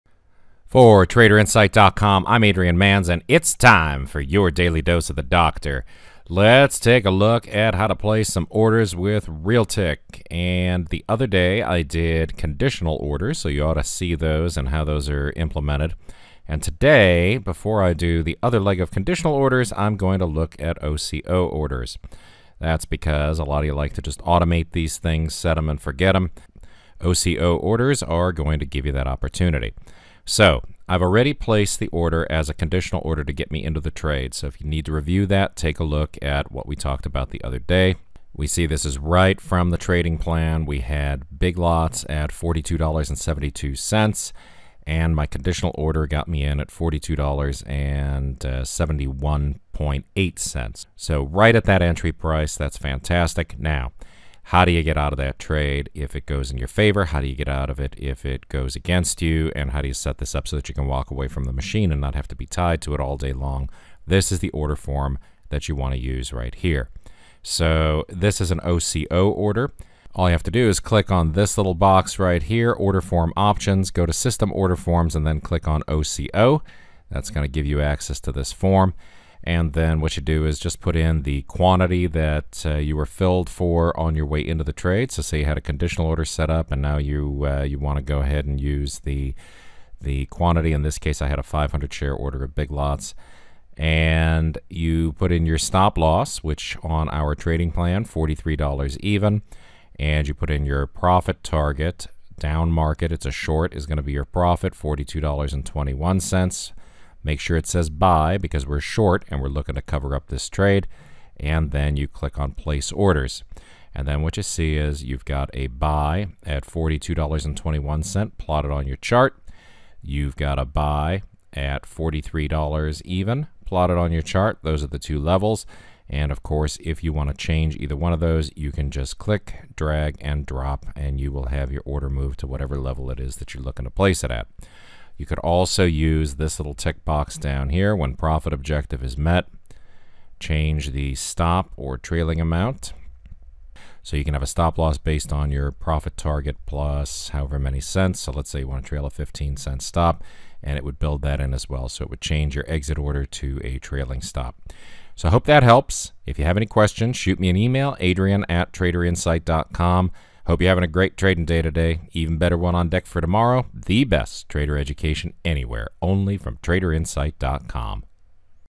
RealTick OCO Orders
If you manually entered a trade, or used a conditonal order to trigger an entry, you canstill set up the equivalent of a bracket order for the exits.  RealTick OCO orders can free you up to walk away from the computer when you want to.